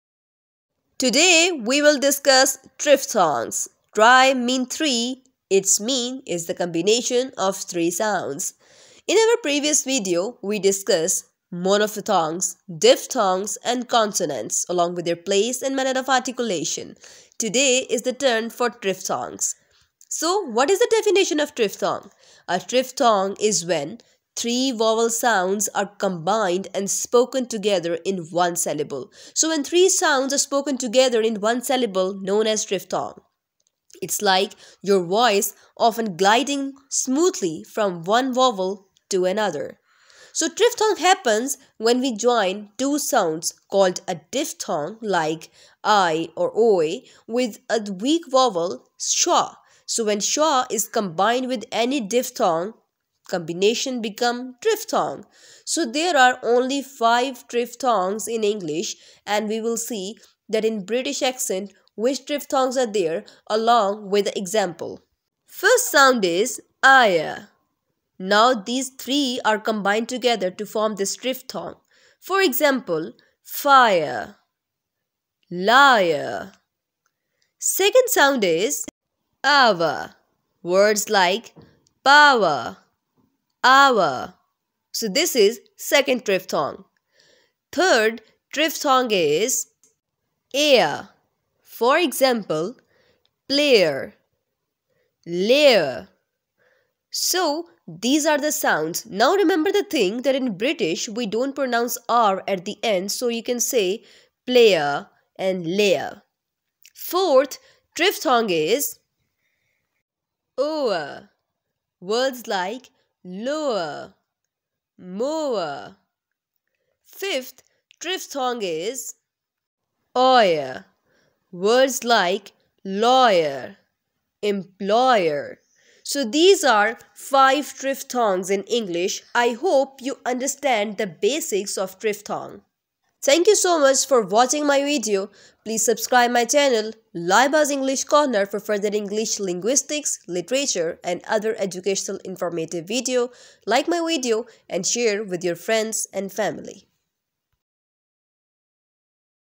Triphthongs | Triphthongs symbols and pronunciation | Triphthongs with examples in Phonetics
Playlist list of Phonetics and Phonology

Watch Complete Course of Phonetics 👇🏻